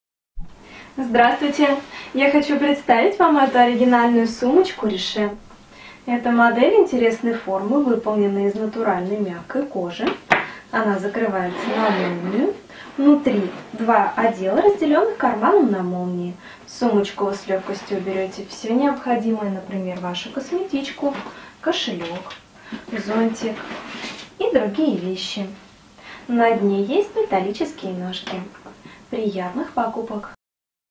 Сумка Richet 1816 FI black
Женственная сумочка, выполненная из натуральной кожи, станет идеальным аксессуаром на каждый день. Станет прекрасным дополнением к любому образу. Украшена серебристой фурнитурой. Закрывается на молнию, носится на плече. На дне имеются металлические ножки.
Внутри: два отдела, разделенных карманом на молнии. На задней стенке карман для документов на молнии.
Снаружи: на задней части вертикальный карман на молнии.